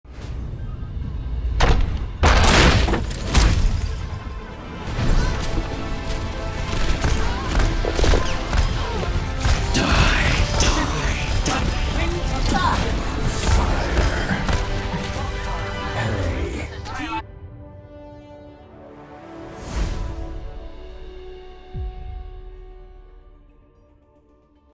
Gib Reaper Plz!
Yes, he's going to be meta, but if you're not going to flank when the team engages then...let me have him!